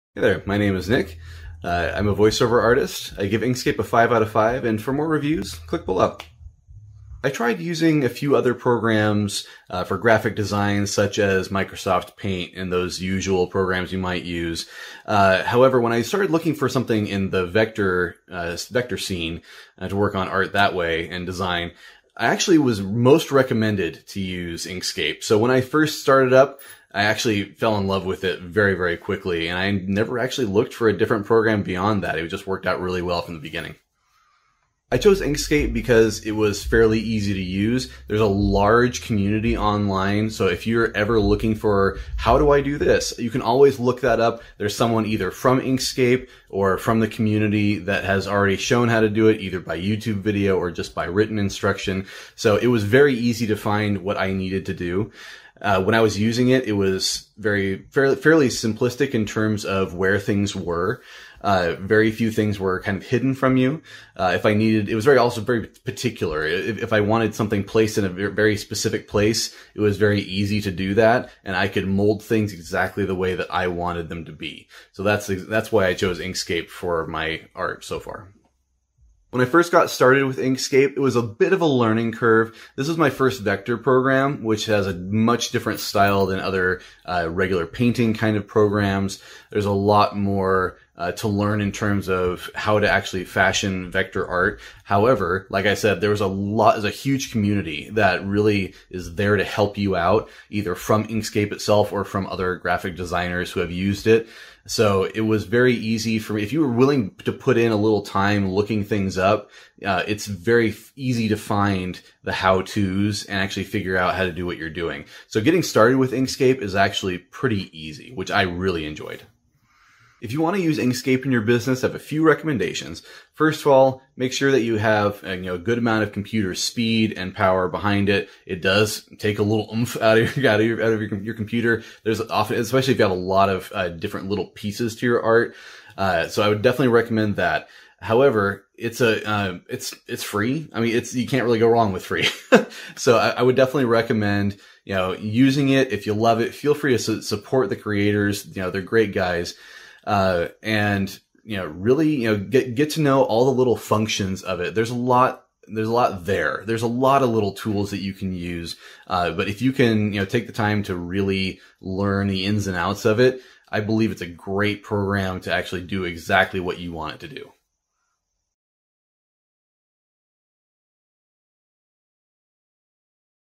Inkscape Review: Used for several projects, all awesome!
💡📘 Interested in Inkscape? Browse Inkscape reviews, pricing and compare with popular alternatives

Inkscape Review 2021.

Business Graphics Software.

Open-source vector graphics tool with object creation and manipulation, rendering, text support, fill and stroke functionality.

0:00 Intro
0:10 What other software products did you consider or switch from?
0:34 What were the reasons you chose Inkscape?
1:19 How easy was it to onboard and integrate Inkscape into your business?
2:00 What recommendations do you have for others considering Inkscape?